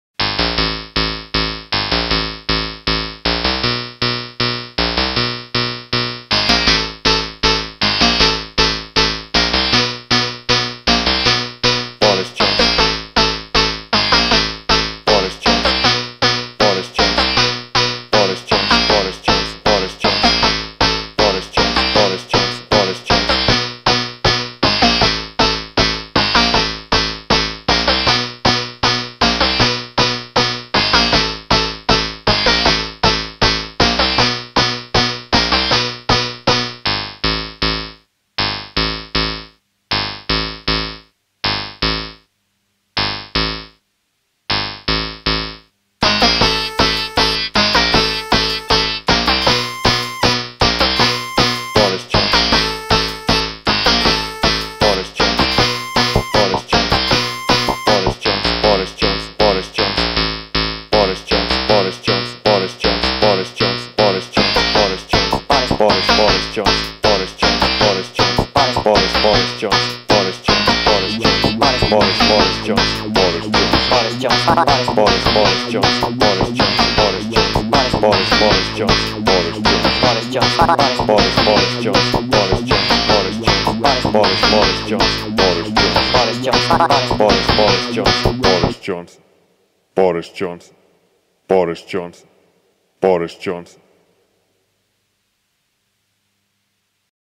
Boris Johnson - an 8-bit tribute
An 8-bit tribute to the ineffable Mayor of London: Boris Johnson. The man who volunteered to be hit with a water cannon after he was banned from using it for safety reasons, the man who rugby tackled a defenceless child, and who single handedly swept up the streets of London after the riots, I salute you.